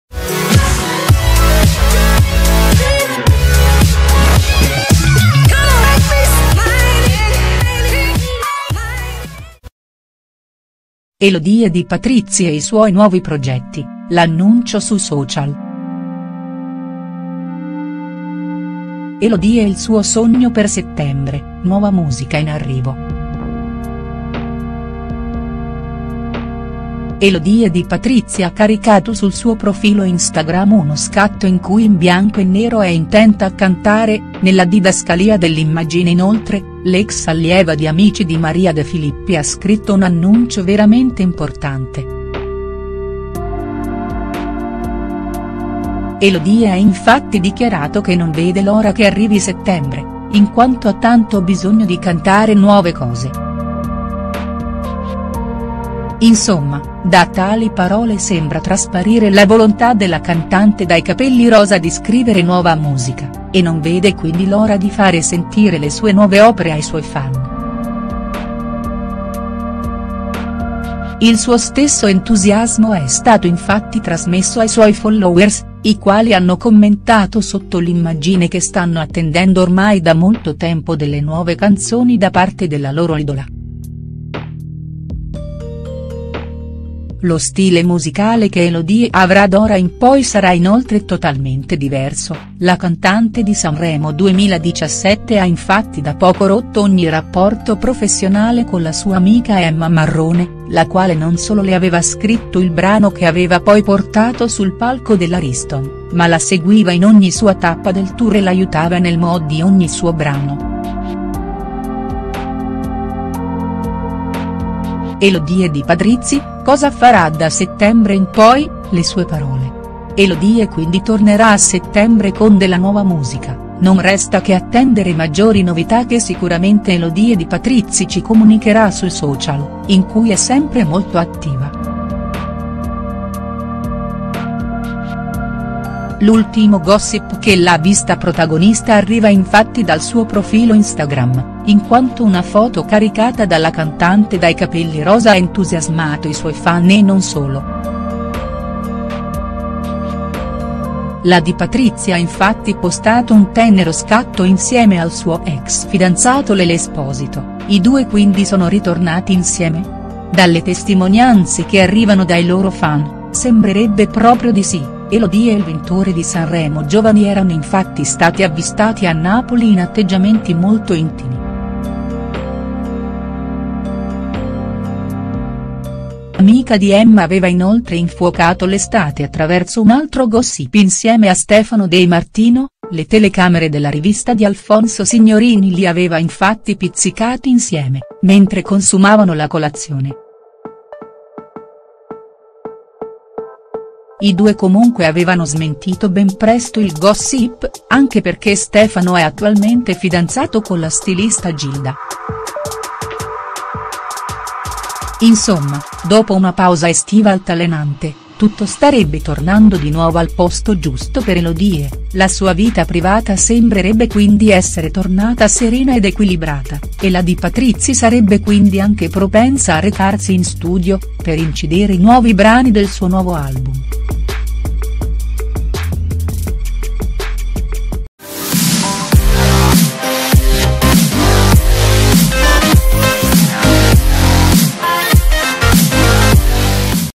Elodie Di Patrizi e i suoi nuovi progetti: l’annuncio sui social.HD
Elodie e il suo sogno per settembre: nuova musica in arrivo Elodie Di Patrizi ha caricato sul suo profilo Instagram uno scatto in cui in bianco e nero è in